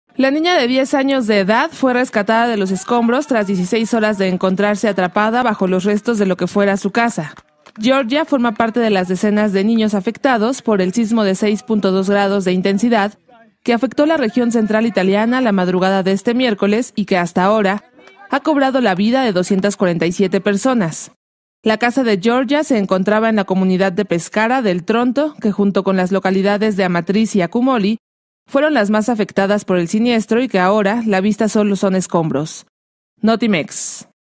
Rescatan con vida a niña italiana, tras sismo en Itaila
Después de 16 horas de estar bajo los escombros de su propia casa, Giorgia fue rescatada con vida.